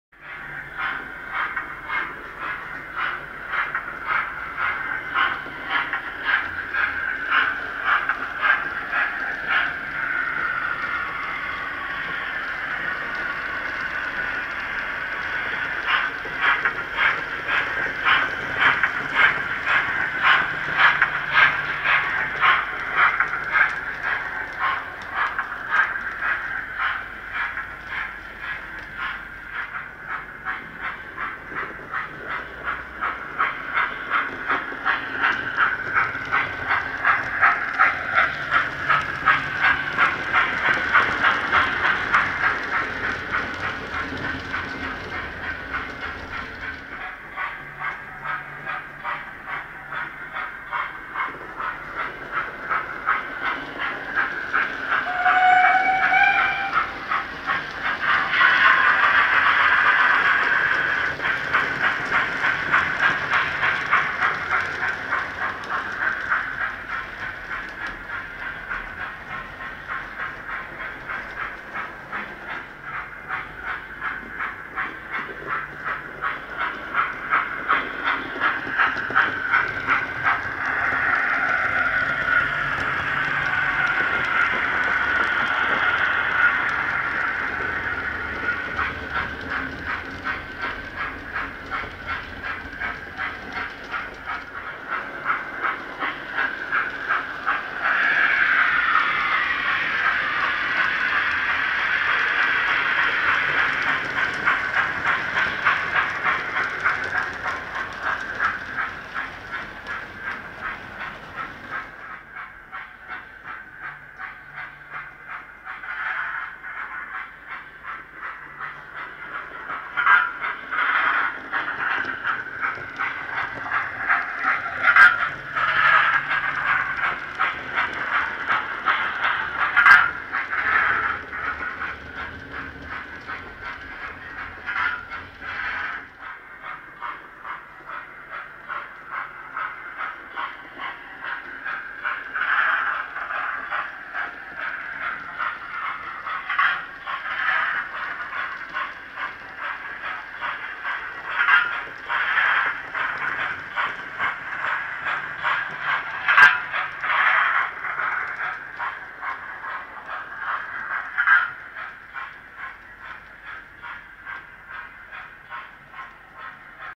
Upgraded Hornby TTS 4F
Hornby TTS 4F with an upgraded speaker which fits in the tender without any modifications. It completely transforms the sound and is available

There are lots of extra  sound functions on this decoder, it's one of the best I've heard. I have tried to use some of these extra sounds in this video but i'm not sure if I've used them all in the right context!

The model has also been weathered, my weathering service can be purchased